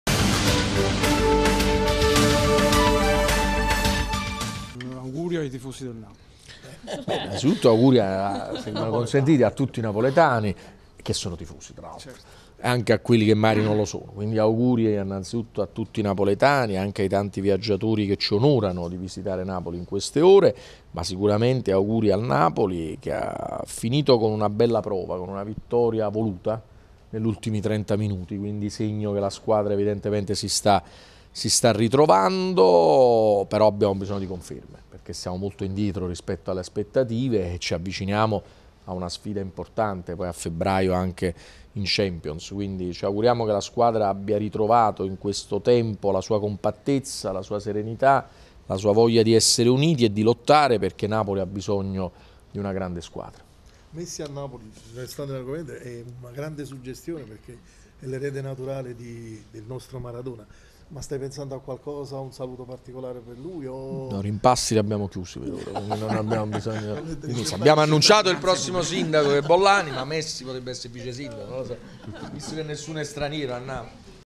De Magistris: auguri ai tifosi del Napoli
Gli auguri del sindaco Luigi de Magistris ai napoletani e ai tifosi.